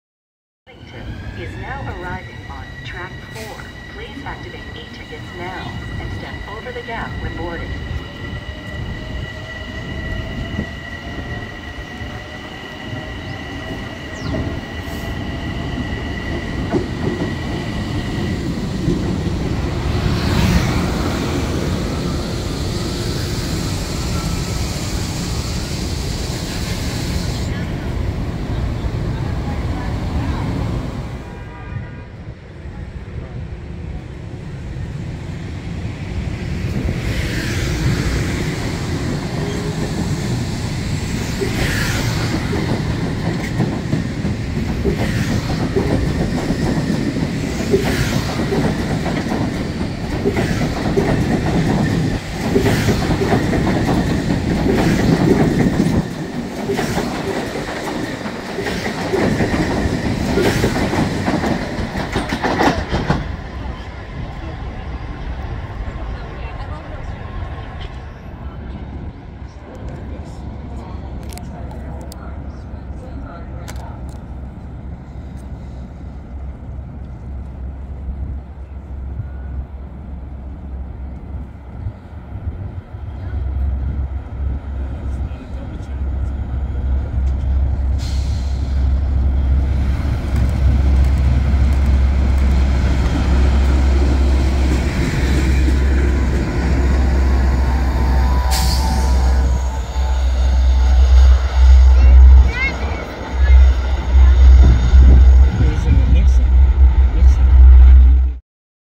LIRR Train 1712 To Huntington Stops At Woodside With 10 Budd M3s & A CE Move • 01/24/2023
a "CE Move" is basically a locomotive swap or one being brought back to the shops. it was EMD MP15AC 172 bringing DE30AC 407 Eastbound towards Jamaica. this set of M3s flew by Woodside earlier in the day on train 1353 and went back out on 1712, in which i happened to be at Woodside for both trains; i saw 1353 from the (7) platform ( only got a photo ) and, obviously, 1712 here. managed to go somewhere else to see some other stuff in between the two catches as well.